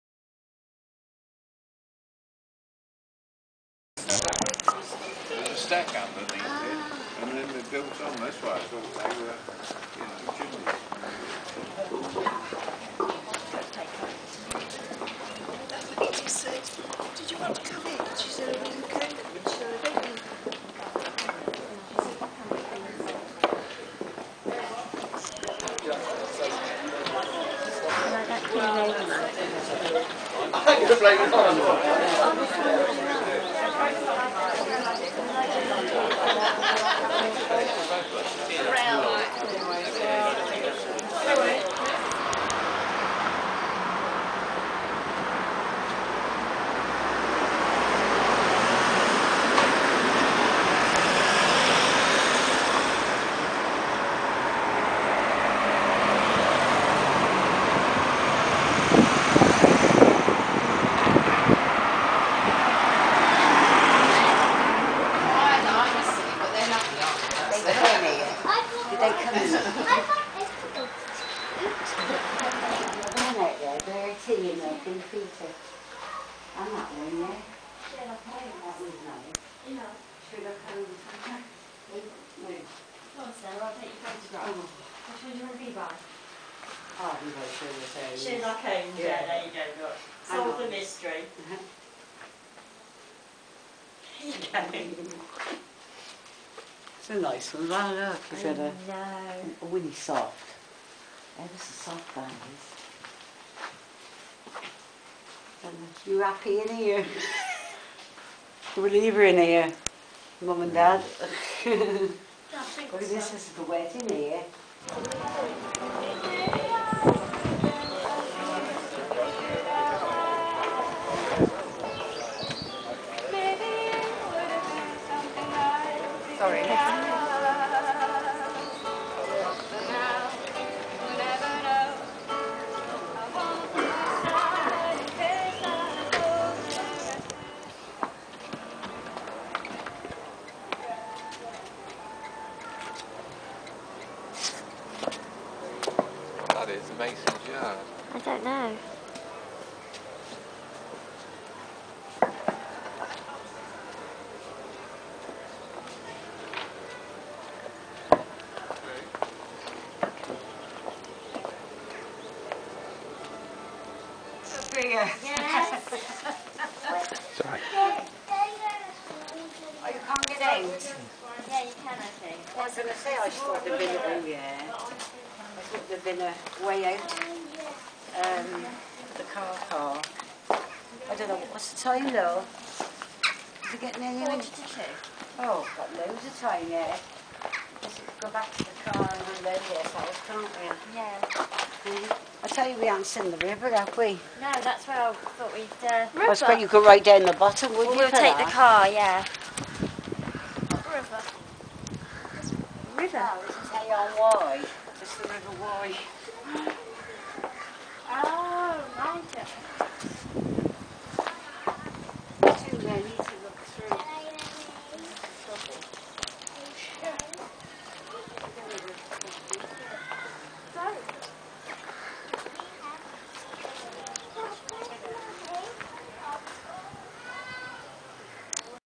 Hay-On-Wye
After an article in my Cath Kidston magazine and the fact that it was one of my late Auntie Sheila's favourite places, we took a trip down to Wales to the beautiful Hay-on-Wye on Thursday 8th April 2010.